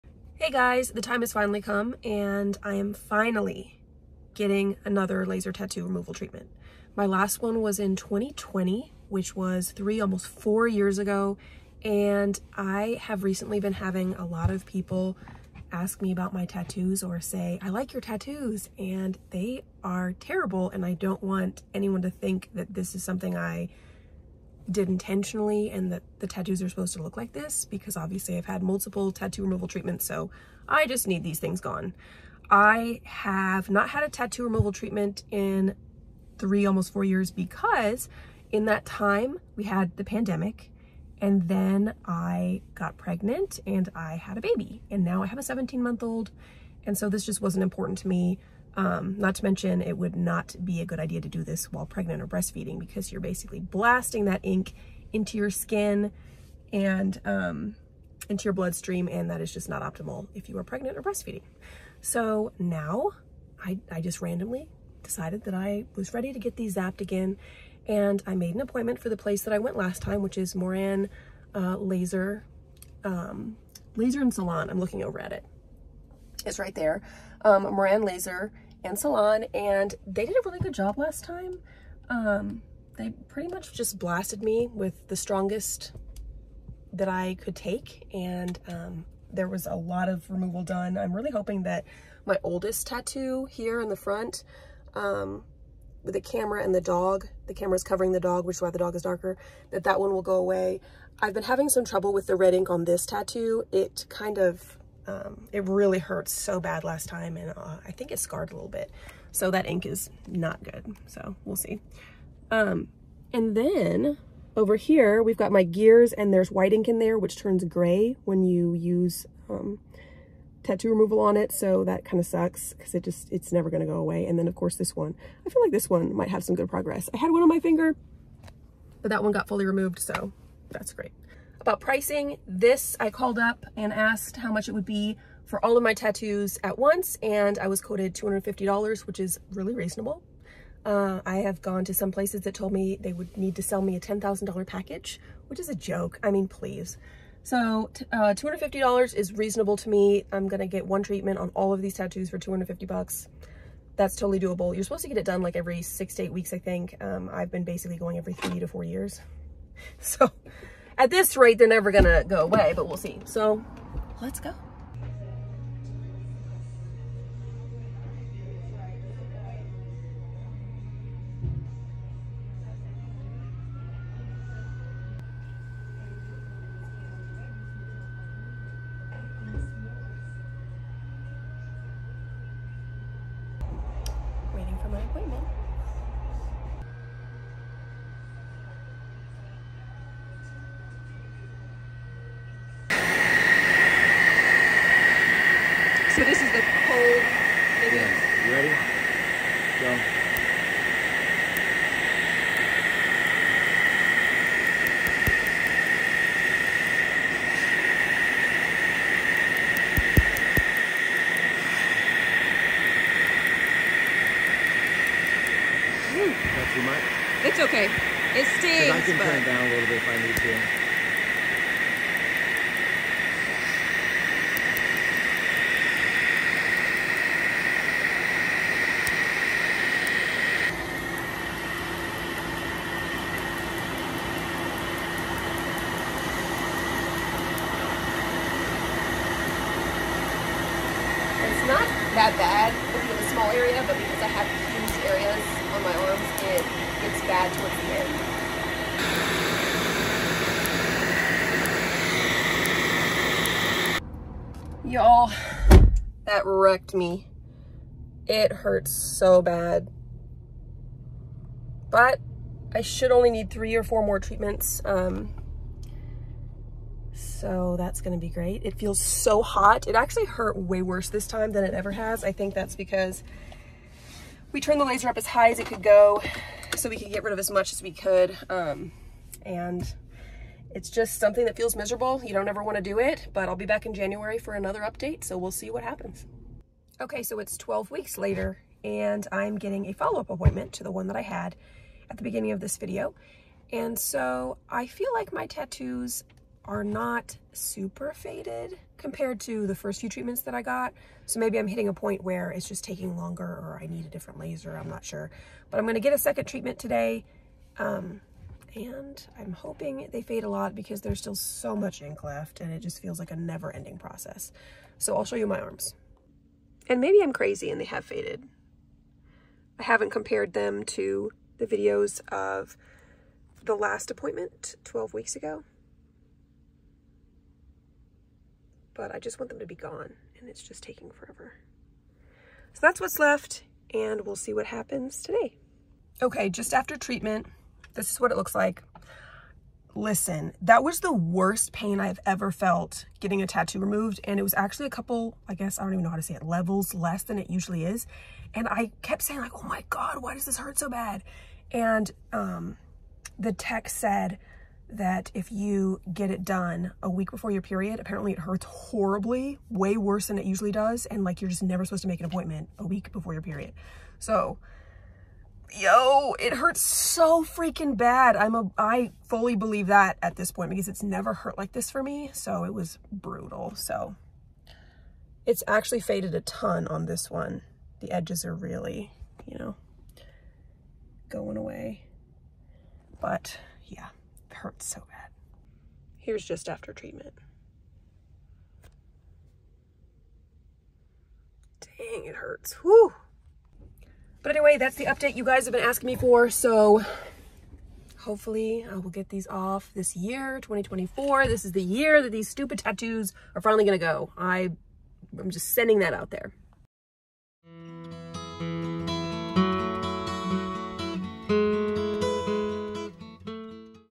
Laser Tattoo Removal Update! See My Appointment + a Surprise
In today’s video, I’m sharing an update on my tattoo removal journey. It’s been several years since I started the process, and I wanted to give you all a closer look at how things are going.

I’ll cover:

🔹 How many sessions I’ve had so far
🔹 The progress I’ve seen with each treatment
🔹 Pain levels and how I’m managing them
🔹 My healing process between sessions
🔹 Tips I’ve learned along the way for anyone considering tattoo removal

 If you’ve been thinking about getting a tattoo removed or are just curious about the process, I hope this video gives you some helpful insights!

Tattoo Removal Shop (love them!)